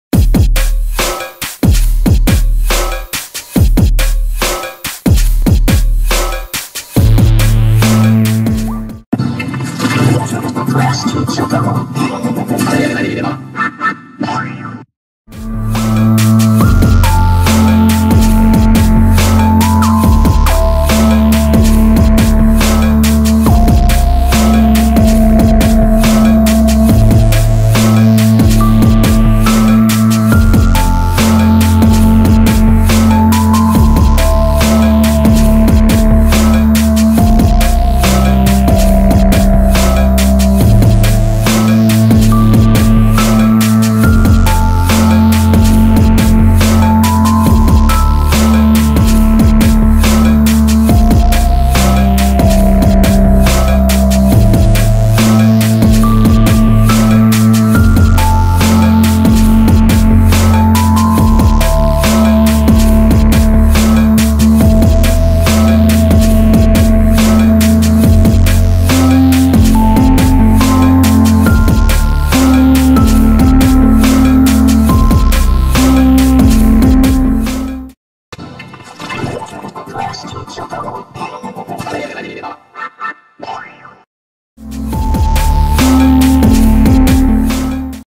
How To Make Fmaj Vocoder 7.0 On Vegas Pro (New Effect)
Made on Friday, August 19, 2022, 12:27:14 pm to 12:27:47 pm

This new effect was made four days ago.

How To Make Fmaj Vocoder 7.0:

(VideoFX)

1st: Gradient Map - Fmaj Vocoder 7.0

[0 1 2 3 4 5**]

or

0; R: 255, G:  255, B: 255, A: 255
1; R:  71, G:    0, B: 165, A: 255
2; R:  30, G:  234, B: 255, A: 255
3; R: 150, G:    0, B:  45, A: 255
4; R:   0, G:    0, B:   0, A: 255
5; R: 255, G:  255, B: 255, A: 255

(AudioFX)

1st: Orange Vocoder - Fmaj VOCODER 7.0.

After choosing "Fmaj VOCODER", let's customize the adjustments below.

Keyboard Key Set:

1st White Key
3rd White Key
5th White Key
7th White Key
9th White Key
11th White Key
13th White Key

Effect by Lynn Grantz

NEW EFFECT Klasky Csupo in Fmaj Vocoder 7 0